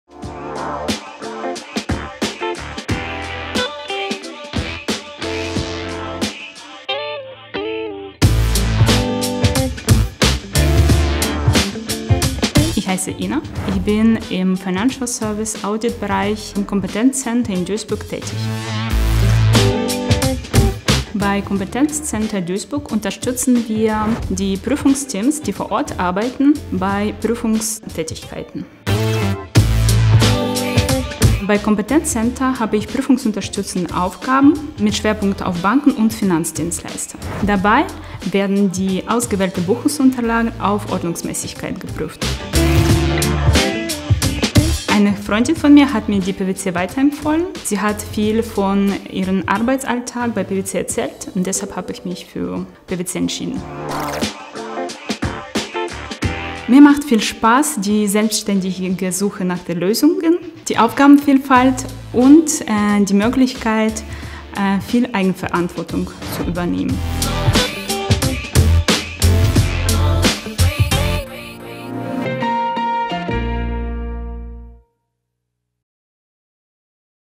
Competence Center Duisburg – Inna
Das Competence Center Duisburg wurde von PwC gegründet, einem der weltweit führenden Wirtschaftsprüfungs- und Beratungsunternehmen. Unser Ziel ist die Unterstützung bei Prüfungstätigkeiten, die nicht vor Ort beim Kunden ausgeführt werden müssen. So steigern wir die Qualität und Effizienz der prüfungsrelevanten Projektarbeit bei unseren rund 6.000 verschiedenen Kunden.

Erfahre mehr über Innas Arbeit im CC Duisburg.